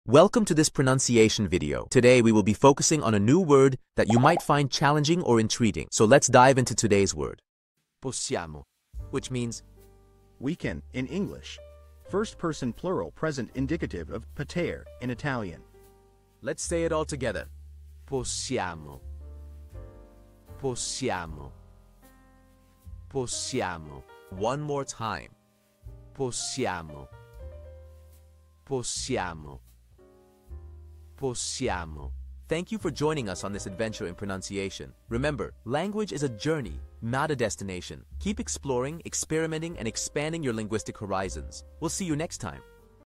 How to pronounce Possiamo
Master the Pronunciation of 'Possiamo Which means we can' - which means : "We can" in English;First Person Plural present indicative of "Power" in Italian. 📝 with @PizzaPronunciation 🎓🗣️ - [Your Guide to Italian]

Learn to pronounce 'Possiamo' perfectly in Italian with our easy and engaging tutorial by @PizzaPronunciation! 📖📽️ In this comprehensive video, we provide you with native speaker examples 🗣️👂 and phonetic breakdowns 🎵 to ensure you get the pronunciation just right.

How to say We can in Italian